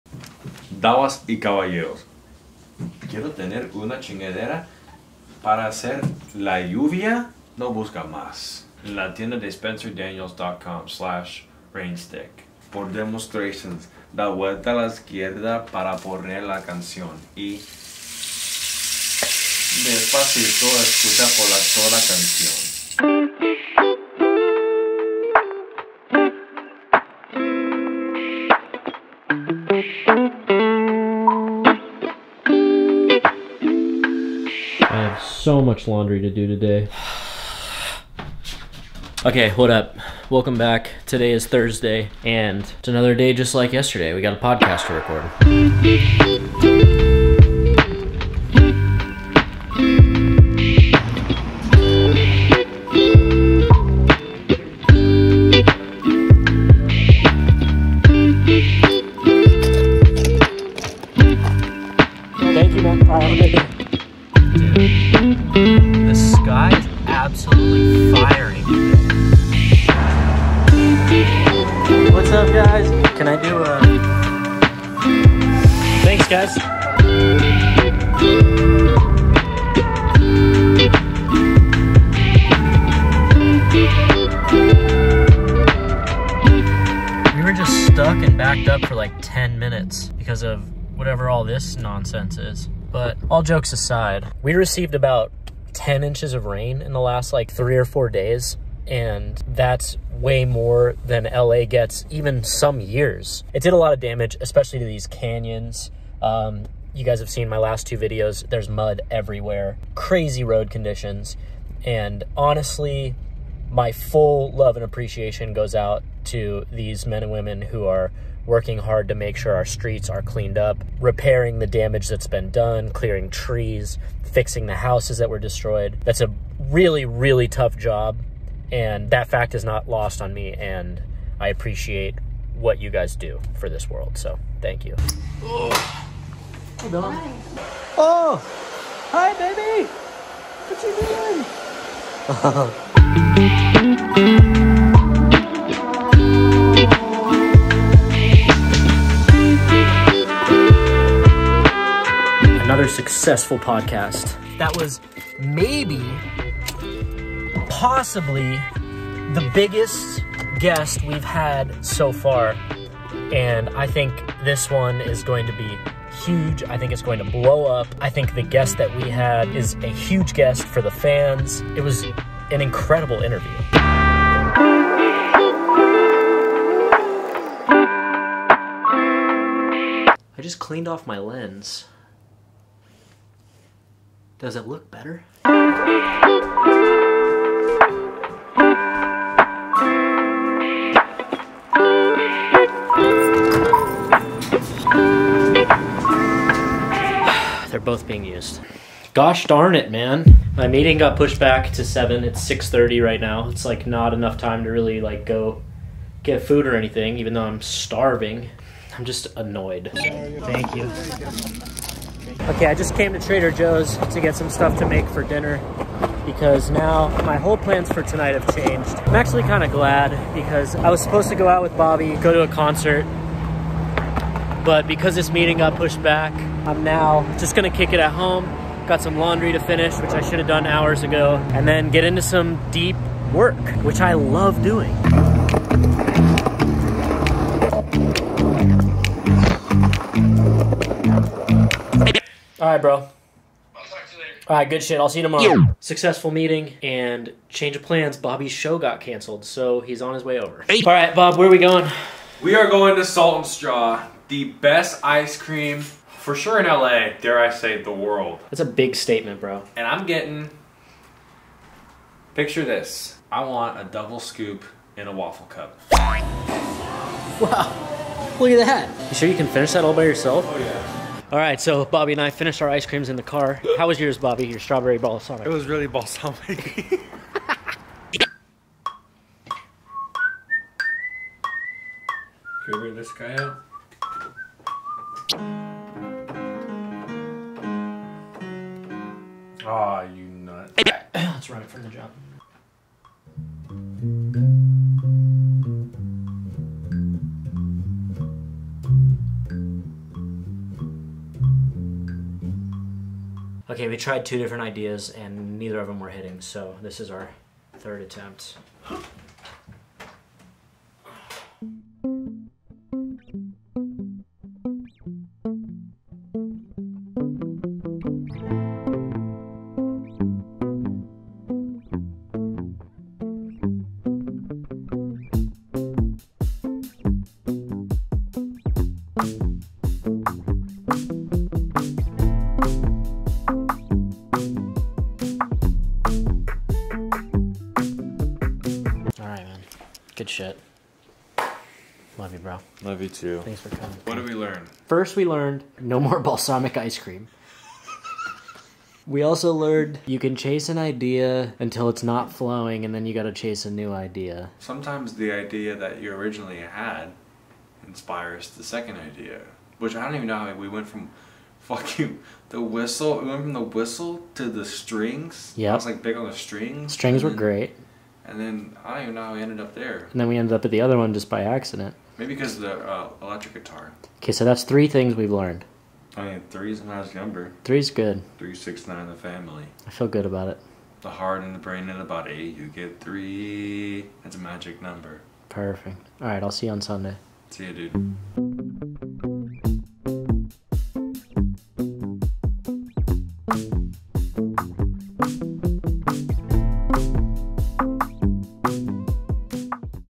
Thank You Construction Workers!!!
In this vlog, I give a shoutout to the construction workers who have been working nonstop to repair the roads and homes damaged by the 10 inches of rain we received in LA the past few days.